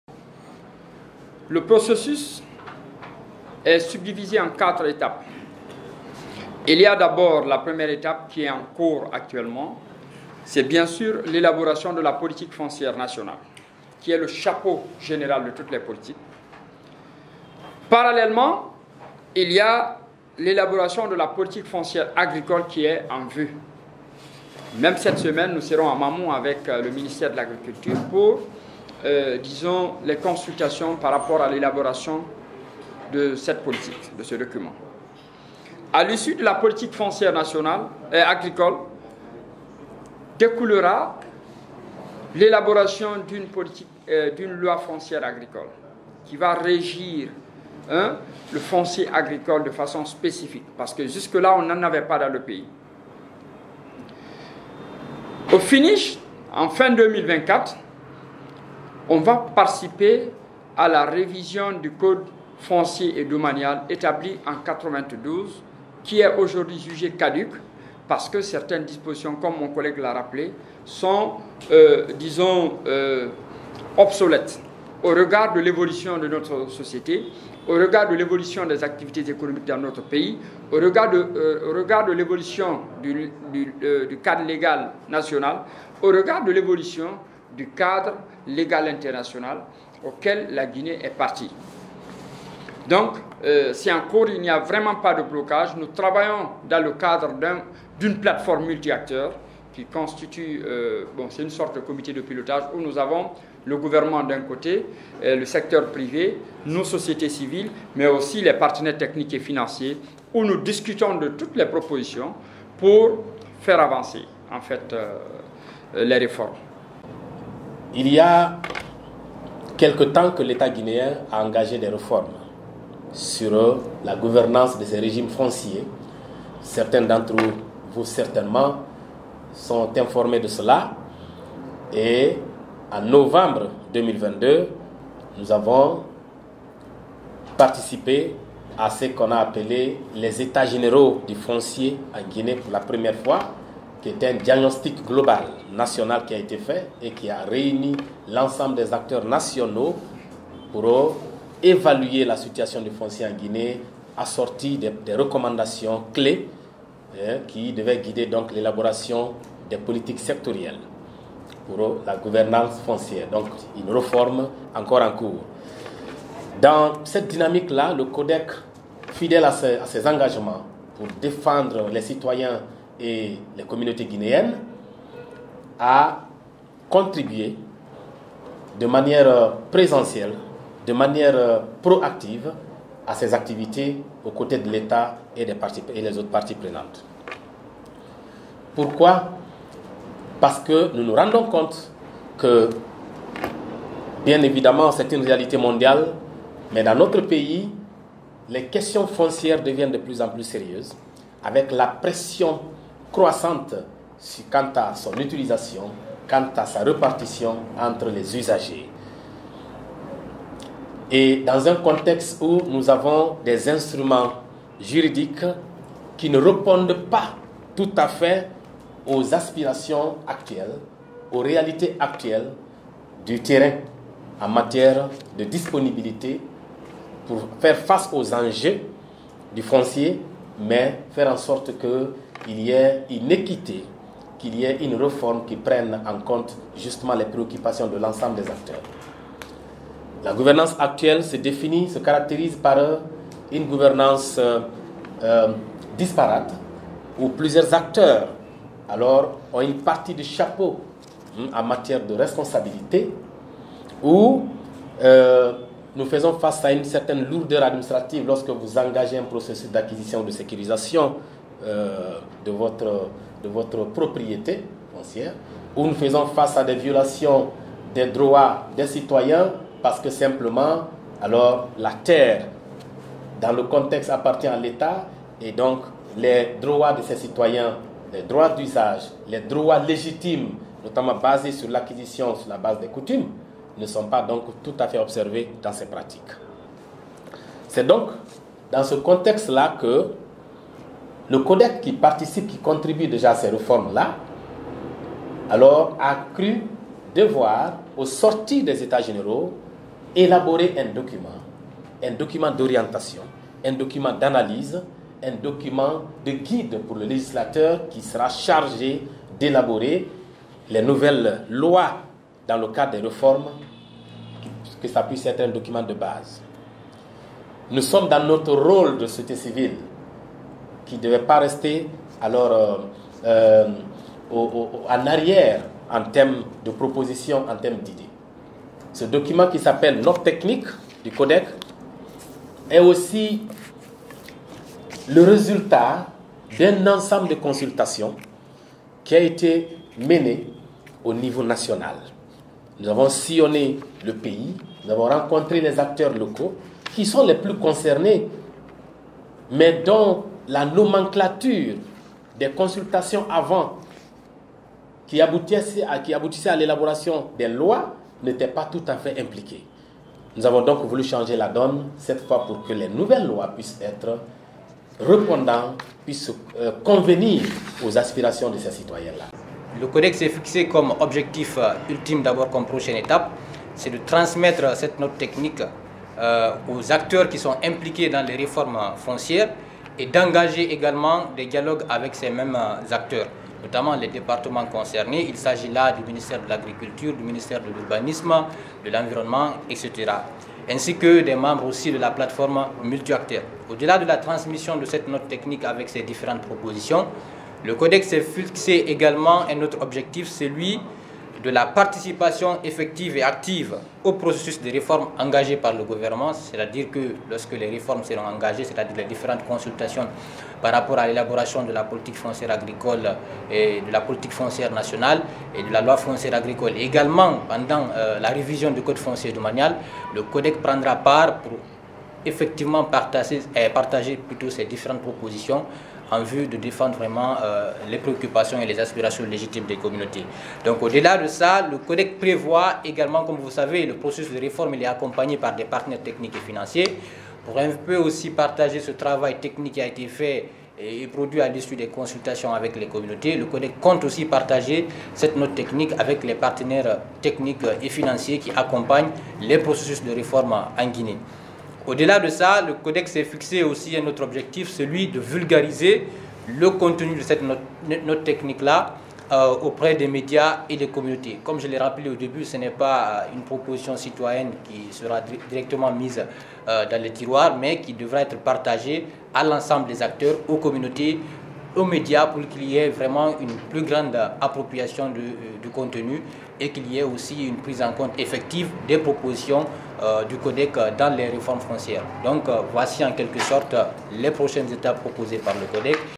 Gouvernance foncière: le CODEC publie les résultats de ses consultations et fait des recommandations
Le collectif des Organisation de la société civile pour la défense des droits des communautés impactées par les projets de développement en Guinée (CODEC) a publié les résultats de ses consultations portant sur la gouvernance foncière en Guinée ce vendredi, 1er décembre 2023, à son siège à Enco 5.

Le Coordinateur du CODEC, Amadou Bah et ses collaborateurs, au sortir des états généraux du foncier, tenu en novembre 2022, ont élaboré un document d’orientation, d’analyse et de guide pour le législateur chargé d’élaborer des lois dans le cadre des réformes du secteur foncier.

Ces consultations menées de mai à novembre 2022, pour la 1ère phase, ont permis de rencontrer et d’échanger avec les différents acteurs concernés à travers tout le pays, a soutenu Mamady Koïvogui, le Coordinateur adjoint du CODEC devant la presse, dont Guineematin.com, à travers un de ses journalistes.

Mamady Koïvogui, Coordinateur adjoint du CODEC

Dans son intervention, le Coordinateur adjoint est largement revenu sur le contexte marqué entre autres sur une ggouvernance foncière disparate, centralisée, une lourdeur administrative des organes de gestion, des procédures complexes et coûteuses, un cadre légal et réglementaire incomplet,  incohérent et une récurrence des conflits fonciers, à travers tout le pays.

Au vu de ces problèmes particulièrement préoccupants, le CODEC s'est fixé pour objectif de promouvoir une réforme foncière juste, inclusive, transparente répondant aux aspirations légitimes des populations guinéennes ainsi que des acteurs économiques impulsant un développement harmonieux.

A son tour, Amadou Bah, le Coordinateur du CODEC a expliqué aux hommes de médias, après ces consultations et des échanges très fructueux, les propositions formulées pour permettre au législateur de fonder sa religion et de mieux aborder les débats sur l’élaboration des textes de lois portant sur la révision du Code foncier et une loi sur le foncier agricole.

Amadou Bah, Coordinateur du CODEC

« Pour solutionner ces problèmes qui ne font que se multiplier et complique l’intervention des acteurs, le CODEC propose un cadre juridique reconnaissant expressément les droits fonciers légitimes (individuels et collectifs)  des citoyens guinéens et sécurisant les droits de propriété et d’usage, et un cadre institutionnel de référence unique de gestion du foncier luttant contre le caractère disparate de la gouvernance actuelle (Le CODEC propose une Agence Nationale du Foncier et ses démembrements qui auront la charge de la gestion du foncier, des domaines et du cadastre). En plus, le CODEC propose la création d’organes de gouvernance décentralisés proches des citoyens et inclusifs assurant la participation des acteurs socio-professionnels, des couches vulnérables pour une question de transparence et de redevabilité, des documents de sécurisation accessibles à des coûts raisonnables (abordables) et un cadre de compensation cohérent, juste et équitable protégeant les droits de propriétés et d’usage des citoyens.

Ce travail doit être complété par une politique et une loi foncière Agricole en adéquation avec la politique foncière Nationale protégeant les droits des paysans, petits exploitants contre l’accaparement des terres comblant ainsi le vide  juridique jusque-là existant, adopter des mesures de conservation et de durabilité environnementale protégeant les ressources foncières contre le changement climatique et ses corollaires et adopter un cadre d’aménagement territorial et cohérent préservant les équilibres socio-économiques promouvant ainsi des villes et territoires viables », a dit Amadou Bah, le Coordinateur du CODEC ».

Les prochaines étapes de ce travail bien fouillé sont la transmission de la note technique du CODEC, le dialogue avec les acteurs impliqués dans les réformes, notamment les départements concernés et les autres membres de la Plateforme multi-acteurs.

« Participation effective et active du CODEC aux processus de réforme engagés par le gouvernement (PFN, PFA et LFA, CFD), le partage de la note technique aux partenaires techniques et financiers qui accompagnent les réformes et la vulgarisation du contenu de la note auprès des communautés et des médias », a conclu Amadou Bah.

Le collectif des Organisation de la société civile pour la défense des droits des communautés impactées par les projets de développement en Guinée (CODEC) a vu le jour en mai 2018. Il regroupe un certain nombre d’organisations de la société civile décidées d’accompagner les populations impactées par des projets dans le cadre de la compensation, de l’indemnisation, selon les normes de référence élaborées à cet effet. Ce, en vue d’éliminer sinon d’atténuer les violations des droits des populations affectées par la mise en œuvre de ces projets.